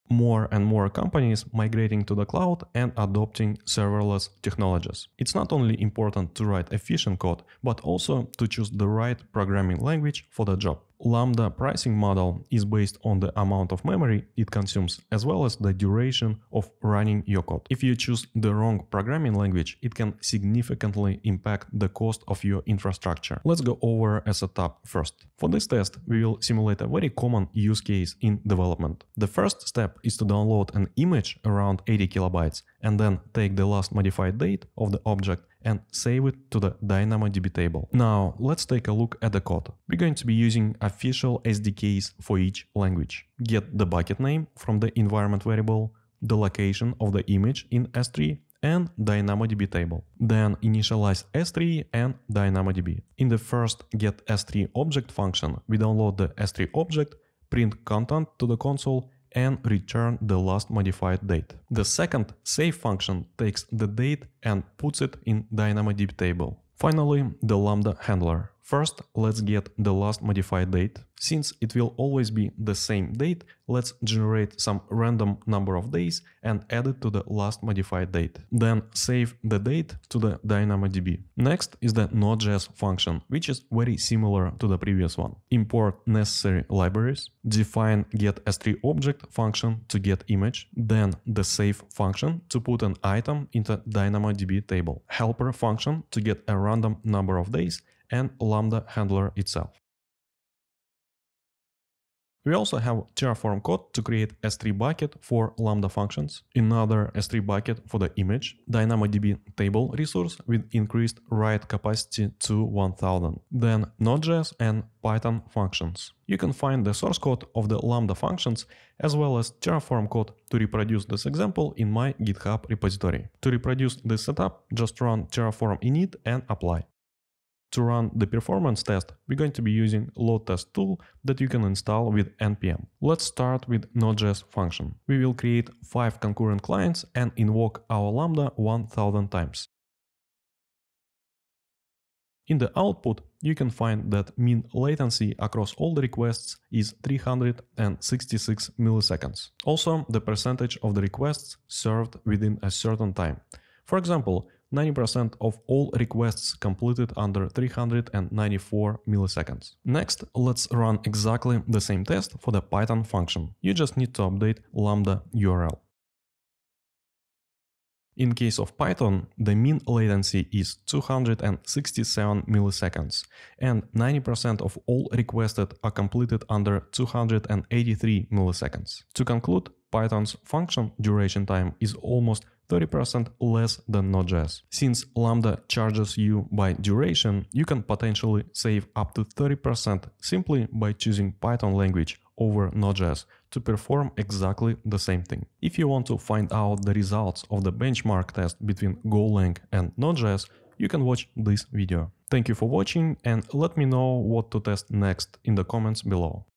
Python vs. Node.js (AWS Lambda) performance benchmark (2023)
⏱️TIMESTAMPS⏱️
0:00 Intro
0:37 Create Python Lambda Function
1:20 Create Node.js Lambda Function
1:40 Terraform Code to Reproduce
2:07 Performance Benchmark